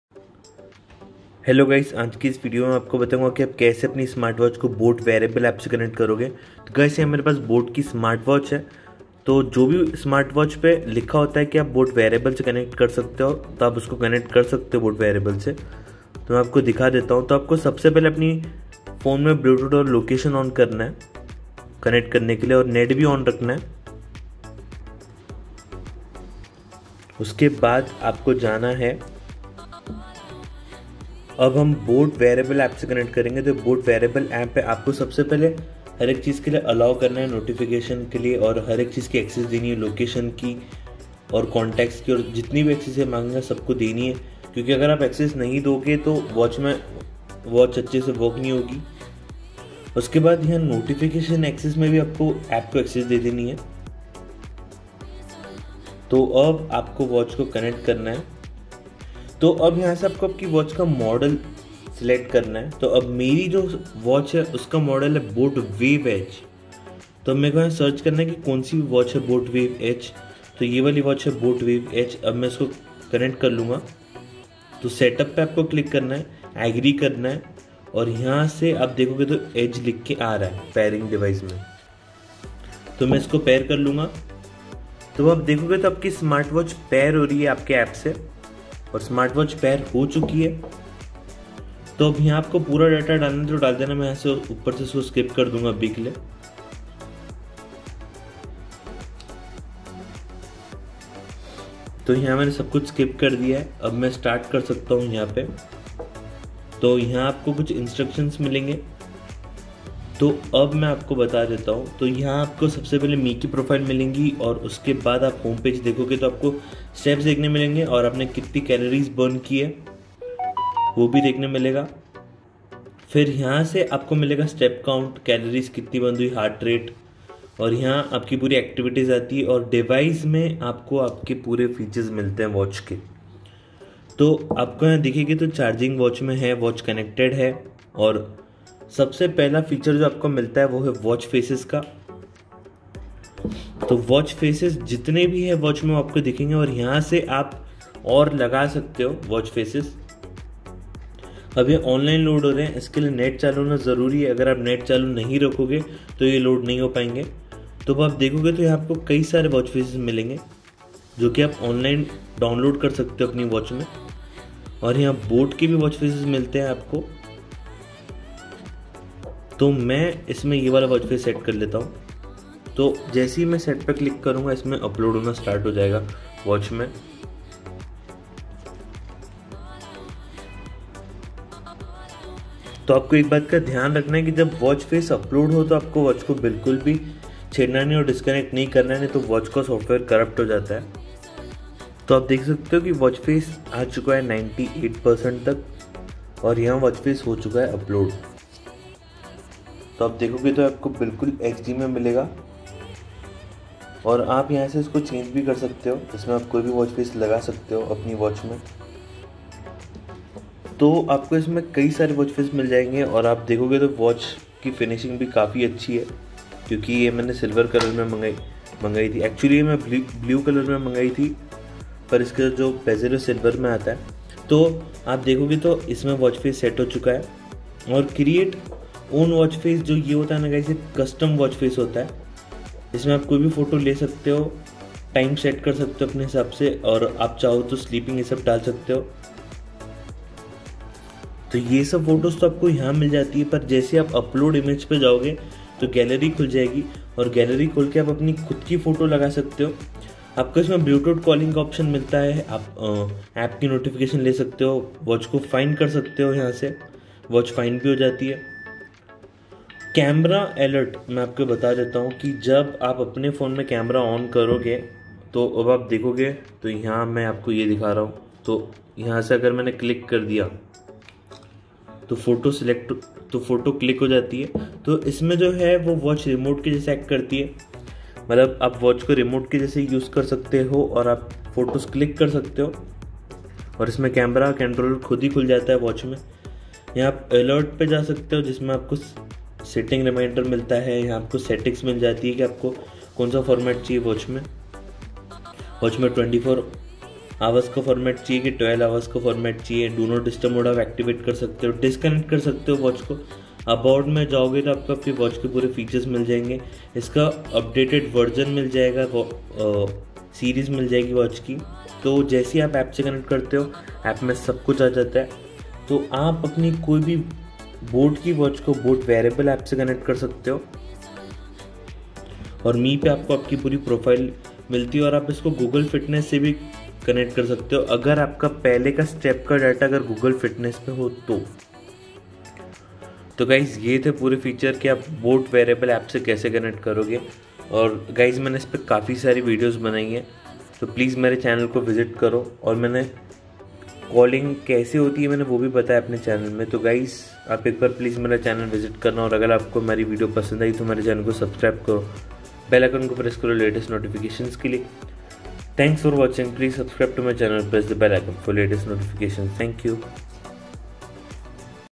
How To Connect With Boat Wearables App | Connect Smartwatch With Boat Wearables App | Boat Wearable
This Video Is about Connect With Boat Wearables App & Features Of Boat Wearables App